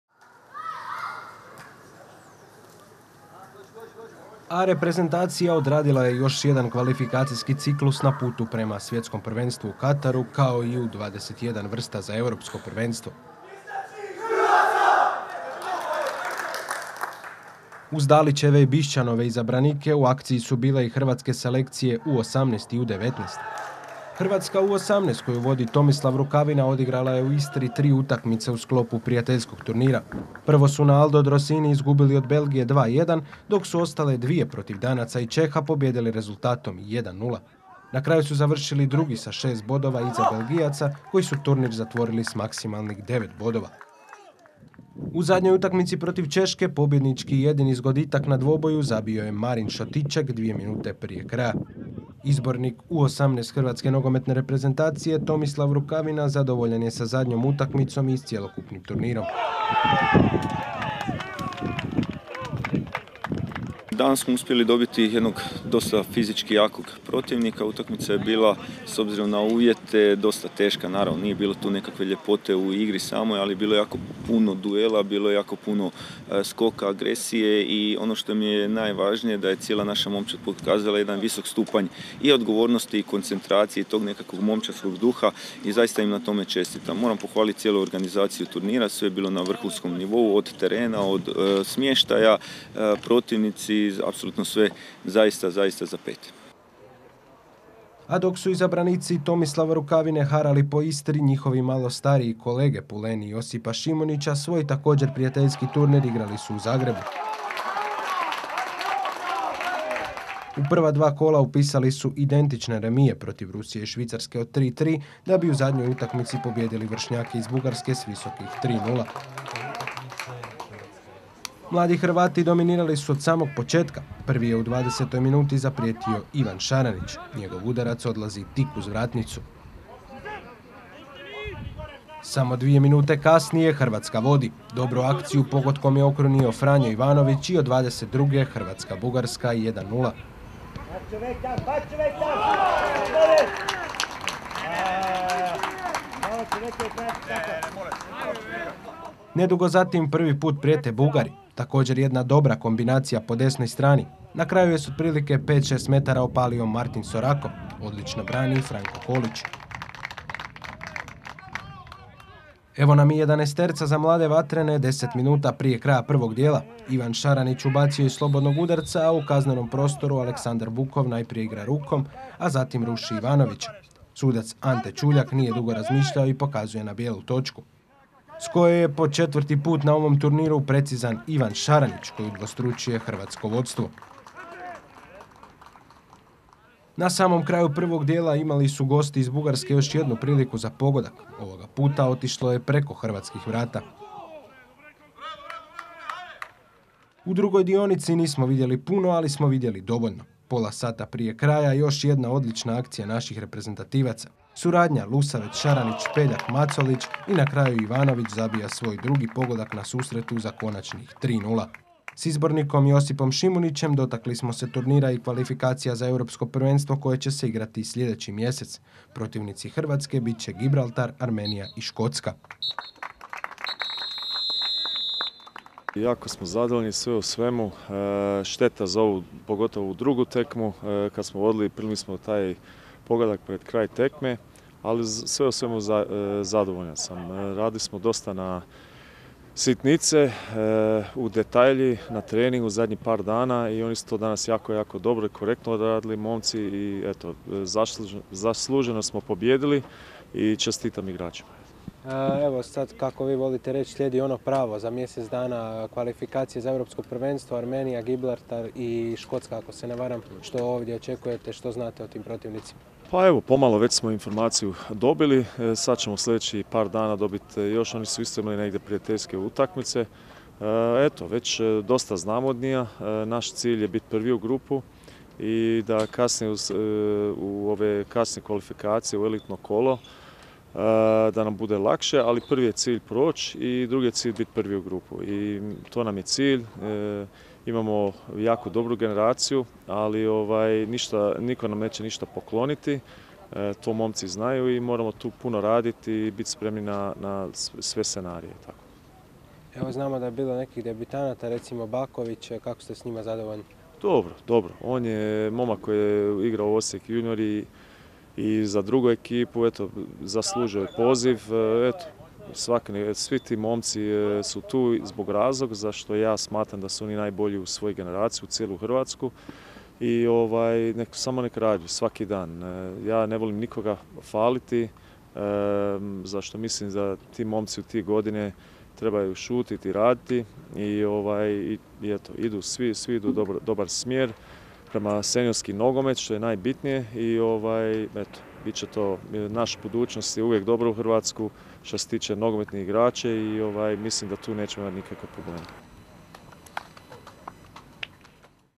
HNTV reportaža: Turniri mladih hrvatskih reprezentacija
Reportaža s turnira mladih reprezentacija na kojima su sudjelovale hrvatska U-19 i hrvatska U-18 reprezentacija.